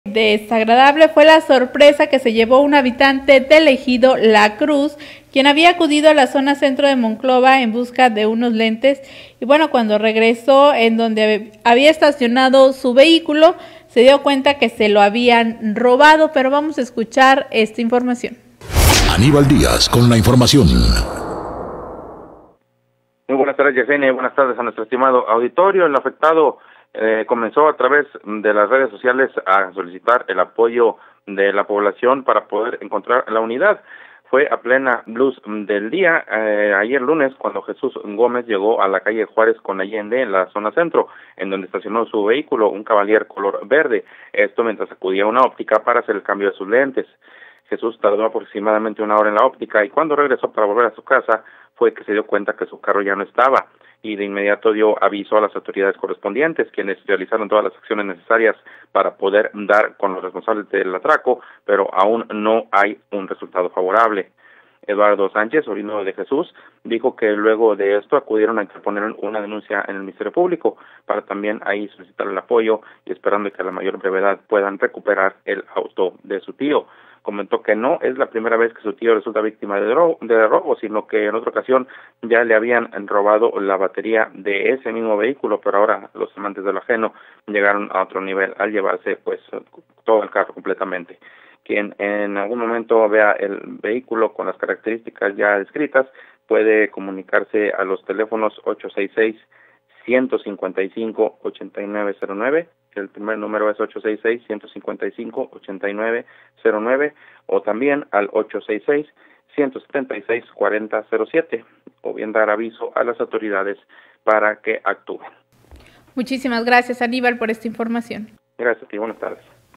Monclova Desagradable sorpresa, cuando regresó en donde había estacionado su vehículo.
Monclova Desagradable sorpresa fue la que se llevó un habitante del Ejido la Cruz, quien había acudido a la zona centro de Monclova en busca de unos lentes y cuando regresó en donde había estacionado su vehículo se dio cuenta que se lo habían robado, informó el afectado Jesús Gómez.